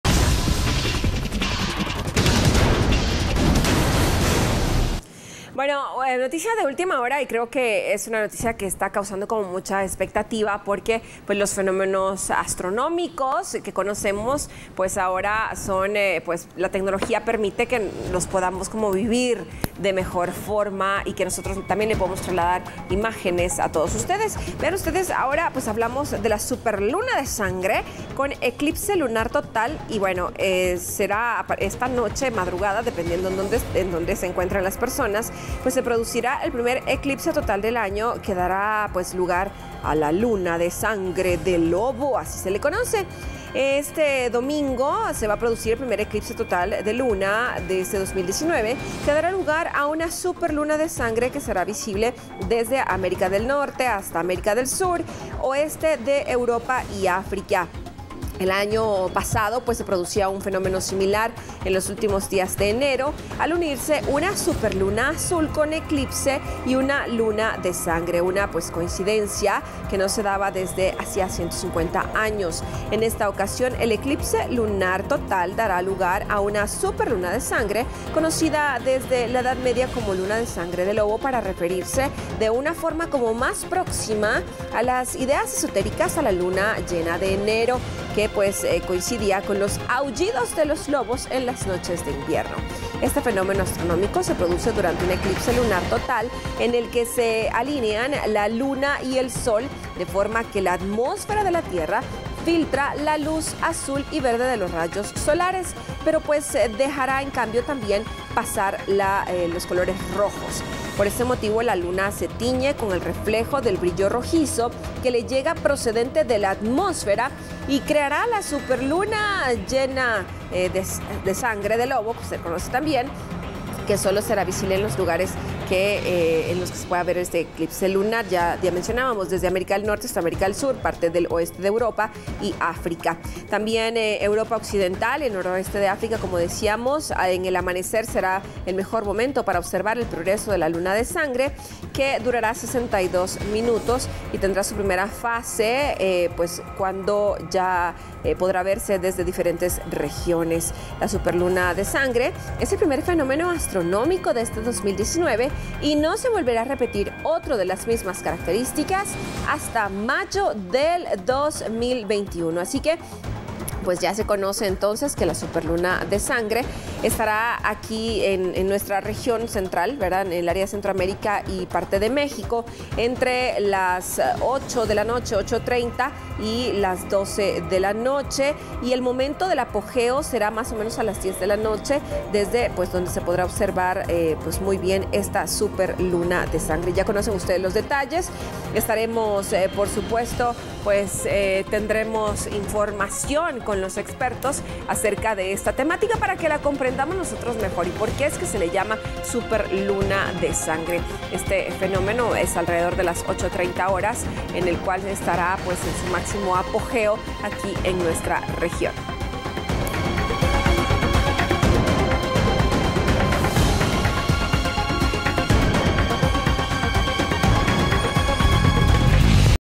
Eclipse total de Luna
Fenómeno se podrá ver este domingo 20 de enero desde las 20:36 horas en Guatemala.